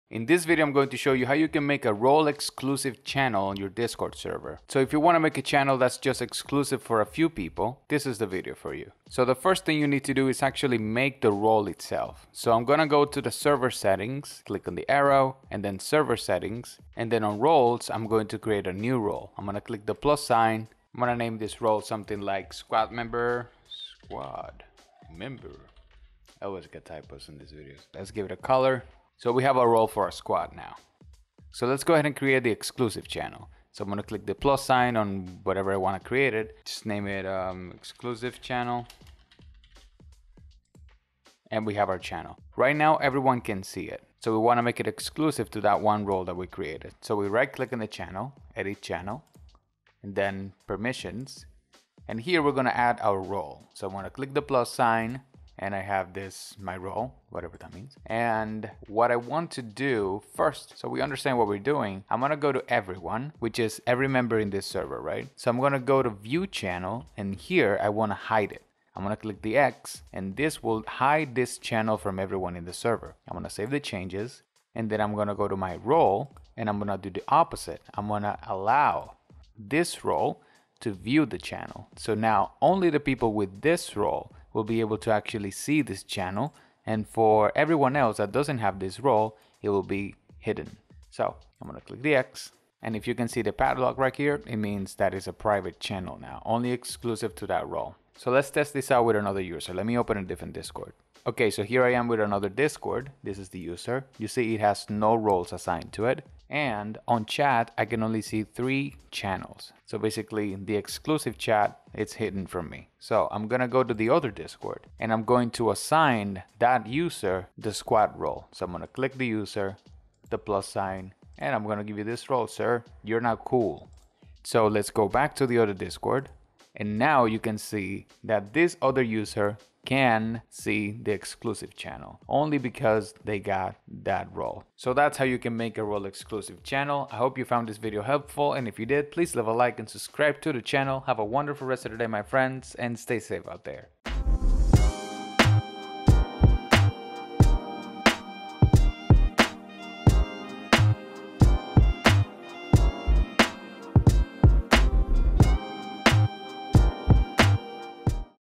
How to Make a Private/Role-Exclusive Channel on Discord
In this video, I show you how you can restrict access to a channel and allow only those with a specific role on your Discord server. This is crucial to be able to have private conversations among a smaller group of people on your server.